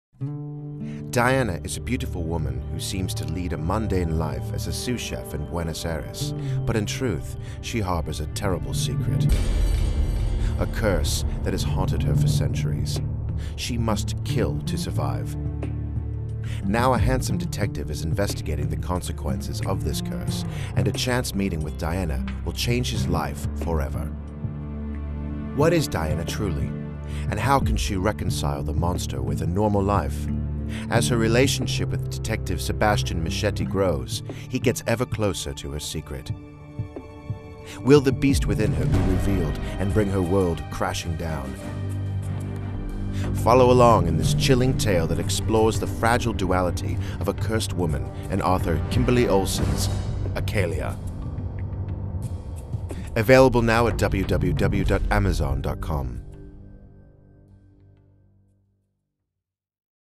Xlibris Book Trailer: Accalia [60 secs]
Accalia by Xlibris Author Kimberly Olsen [60 secs]

Overview:
In antiquity she was born, and on the modern streets she walks. Who is she? What is she? Half woman, half wolf, she represents everything we want to be and would fear ever to be.

She is a wanderer and has lived in many places, but now we encounter her in her current abode of Buenos Aires, Argentina. She uses the name Diana and works as a sous-chef, in order to assimilate herself into normal life. She has many talents, and is gifted with keen insight. In her human form, she has an odd way of being able to help people, but her curse still afflicts her. She must kill to survive. Brutal murders are being discovered around the city, and a man investigating them will cross paths with her in a chance meeting. They encounter each other again, at a Tango dance hall, and neither would ever be the same after that.

She has to be wary about her relationships with humans, but will she let herself go this time? Will she be able to explore human emotion on a deeper level, perhaps putting at risk the life of the very man who manages to open her heart? Will he find out that the woman he has fallen for is the very monster he is trying to catch?

This is a chilling novel, but it also deals with the human psyche, duality, exploring other sides of ourselves, tapping into our senses, a woman's power, vulnerability, love, detachment, mortality, society's hypocrisy, and of course- how appearances can be deceiving. Not only can appearances be deceiving, but often people don't even know themselves.

Written A Book?